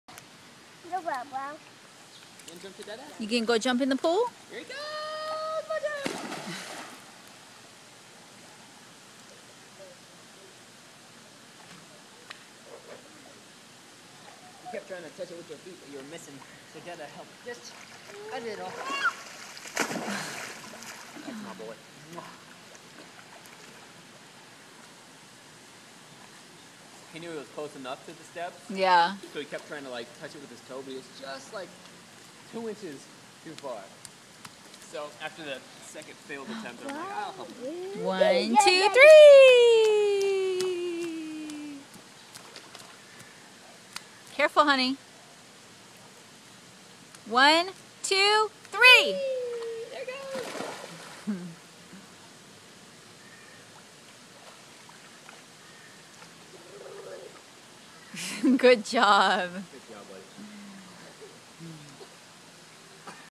Evan is a PRO!
2nd jump in Evan pops up and swims over to the stairs.